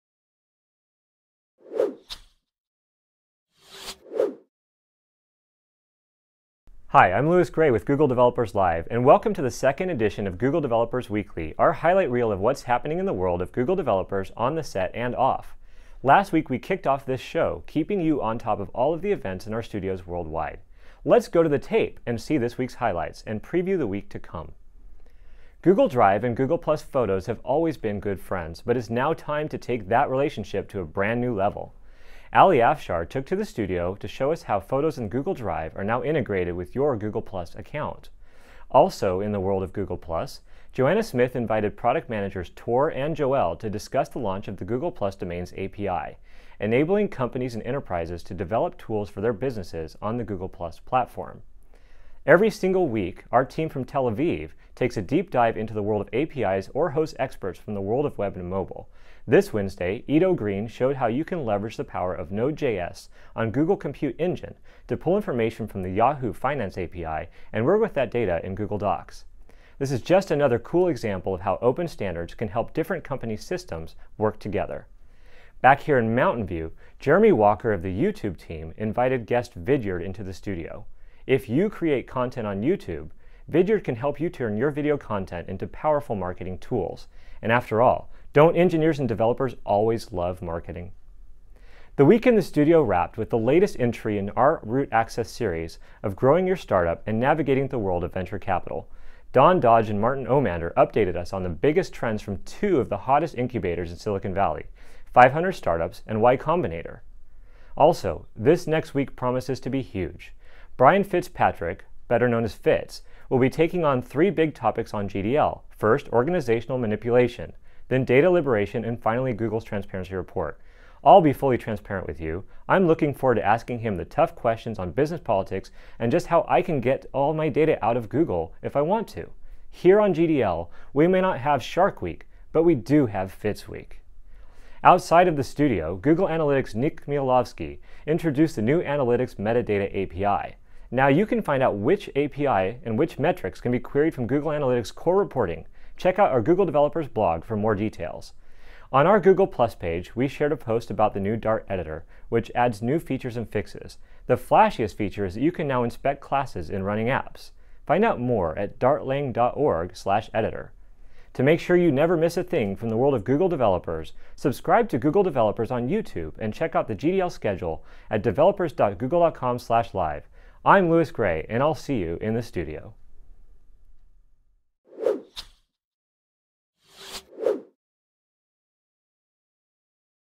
Google Developers Weekly Ep. 02 - August 26th, 2013
The Week in Google Developers Live, with host Louis Gray from Mountain View California.

Root Access: YCombinator and 500 Startups Demo Days, trends and overview
Don't miss Reto Meier's Android Weekly wrapups, including tips on Responsive Design, Animation Techniques and Making Apps Beautiful